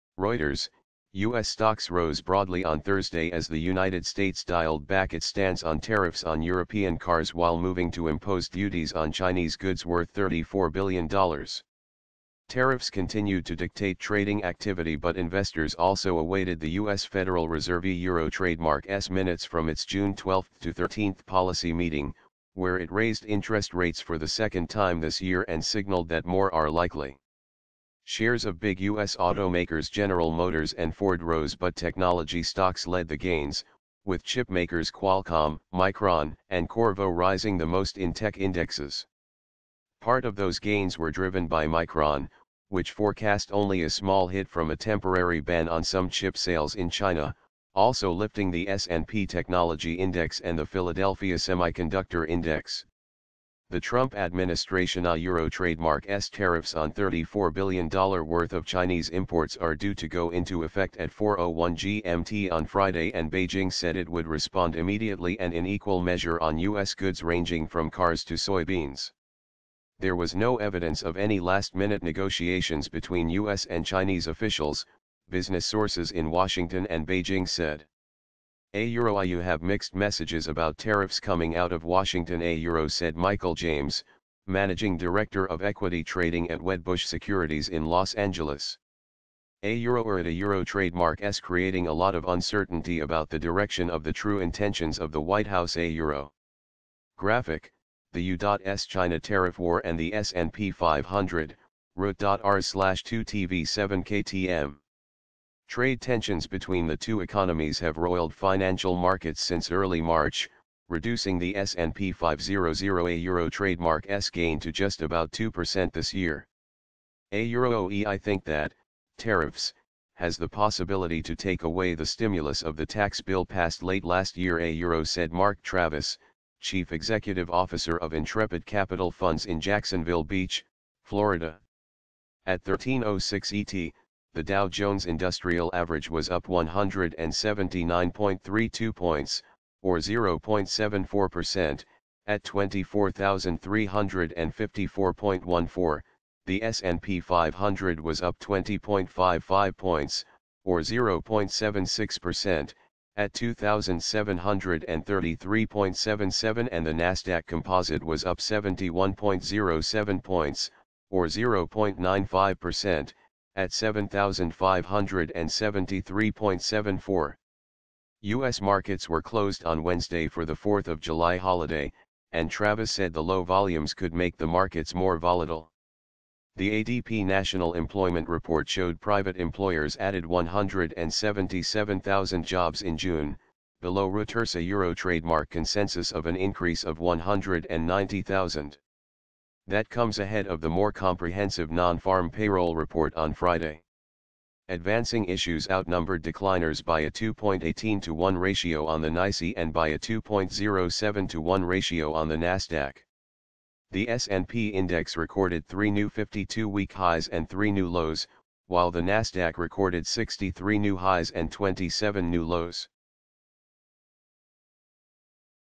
Wall Street Rises As Trade Fears Ease; Fed Minutes Awaited | News Today | 07/05/2018 | Donald Trump
U.S. stocks rose broadly on Thursday as the United States dialed back its stance on tariffs on European cars while moving to impose duties on Chinese goods worth $34 billion.